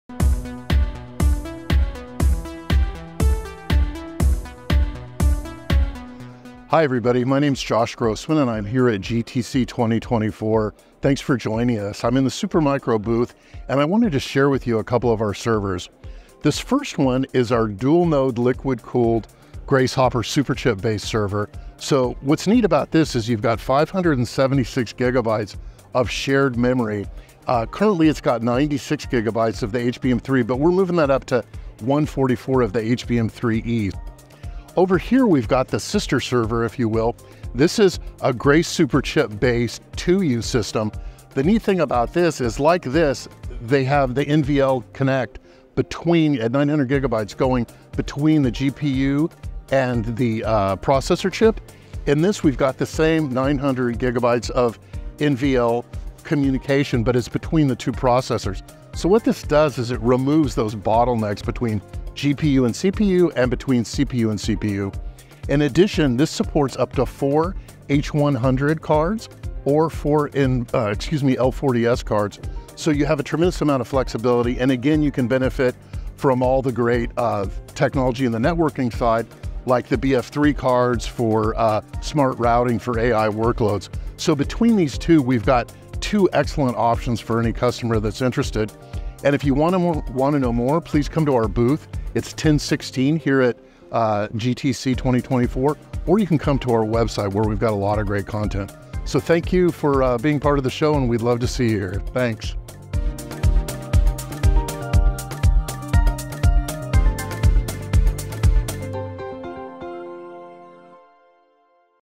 Explore the Supermicro NVIDIA MGX™ Systems at NVIDIA GTC 2024!
Join our Supermicro expert Josh Grossman, Principal Product Manager, as he showcases the Supermicro NVIDIA MGX™ Systems at our GTC 2024 booth.